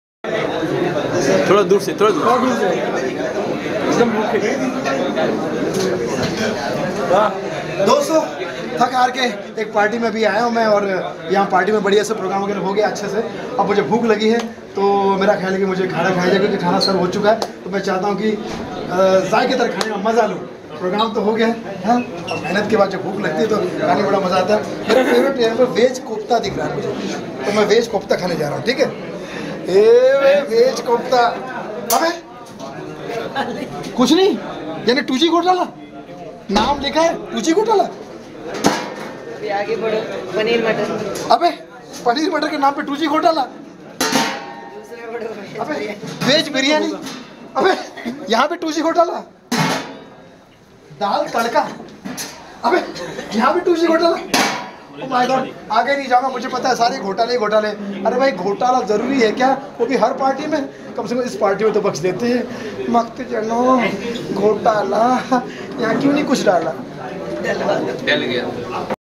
PARTY 2G GHOTALA SAY S SUNIL PAL COMEDY VIDEO
RAJESH SRIVASTAVA FILMS TODAY 2018  COVER PAGE  Issue was Unveiled on 22 DEC 2017.PARTY  WAS ORGANIZED GUEST RANJIT, SUNIL PAL, RASHMI MISHRA,GURUJI, Pahlaj Nihalani,ASHOK SHEKHAR,AMARJEET,RACHANA KHANNA,PUNEET KHARE,MANISHA KHARE,SATISH AGARWAL AND MANY MORE  WERE PRESENT.